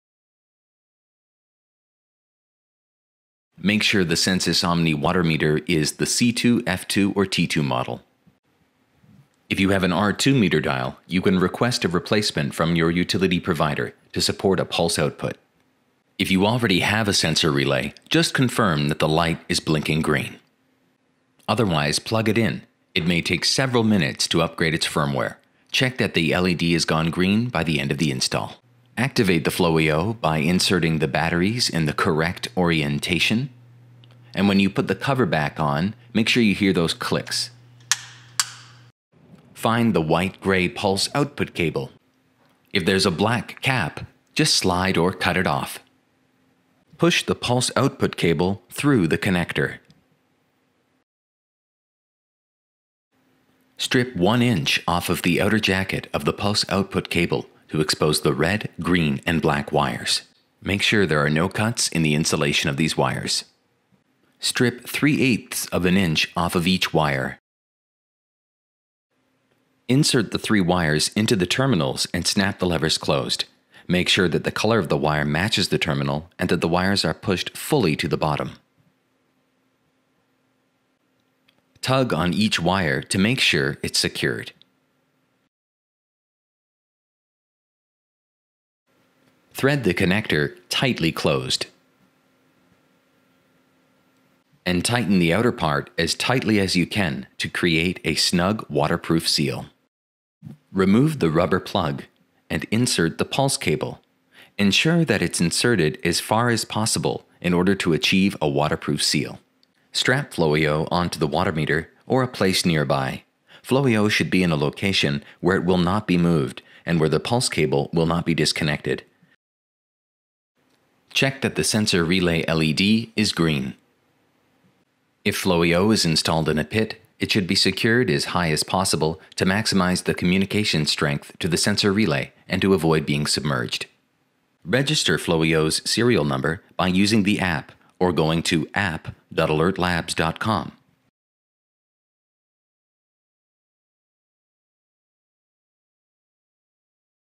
Flowie O Pulse Cable Installation | Alert Labs
Use the Flowie-O Pulse Cable to install the Flowie-O water flow sensor on the following Sensus Omni Water Meters: C2, F2 and T2.